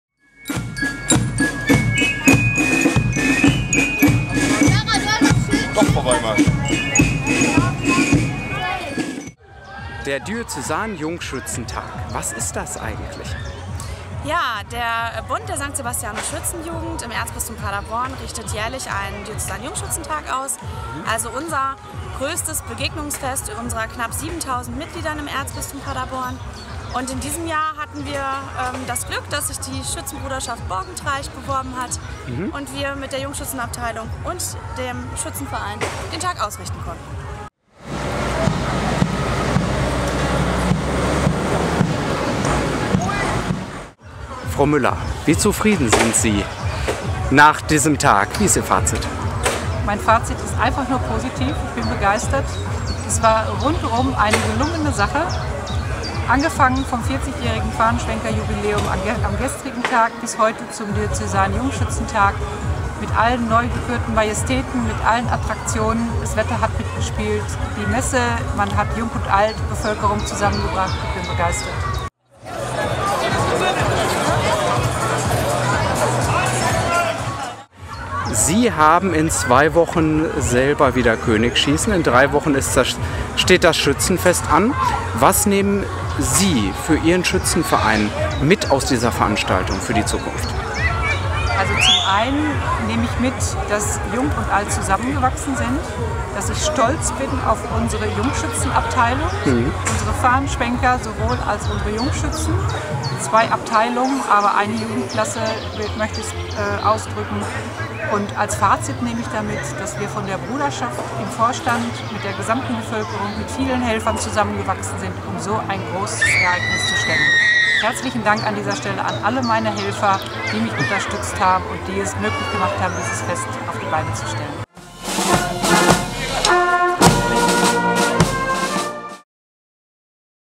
Jungschützentag in Borgentreich
Borgentreich, 14. Mai 2017: Das hat es in der Orgelstadt seit 30 Jahren nicht mehr gegeben: etwa 600 Schützen und Mitglieder von Spielmannszügen und Kapellen haben am Wochenende in Borgentreich den Diözesan-Jungschützentag des BdSJ Diözesanverbandes Paderborn gefeiert. Die ganze Stadt erstrahlte dabei in Schützengrün.